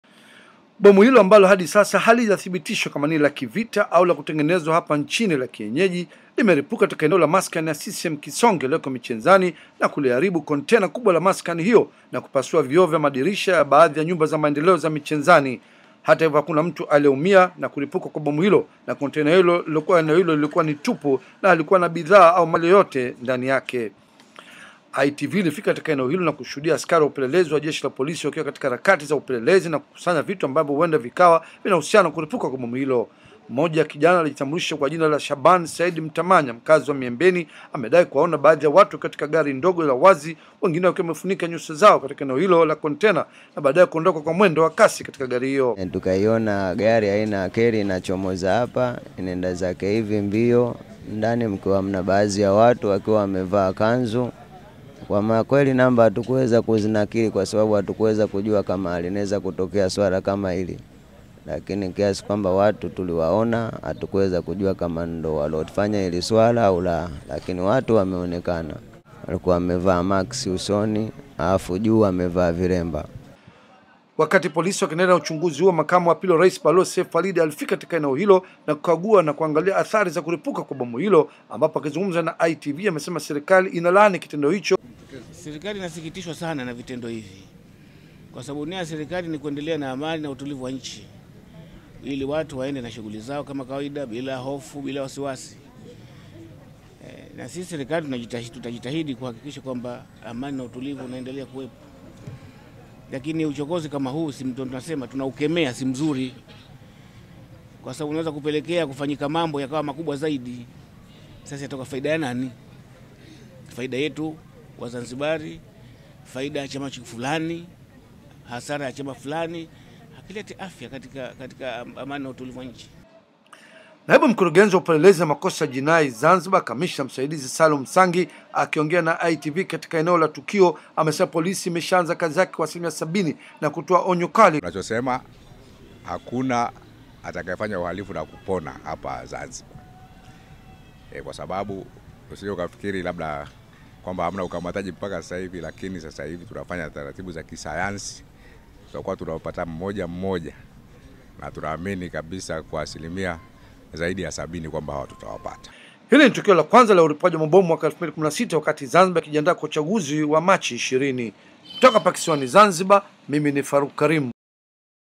Wakazi wa Zanzibar wakumbwa na hofu baada ya bomu kulipuka na kuzusha tafrani.
Kukiwa kumesalia wiki mbili kabla ya marudio ya uchaguzi mkuu Zanzibar  wakaazi wa maeneo ya michenzani na walio karibu na maeneo hayo wamekumbwa na hofu kubwa baada ya bomu kulipuka usiku wa kuamkia leo  na kuzua tafrani kubwa.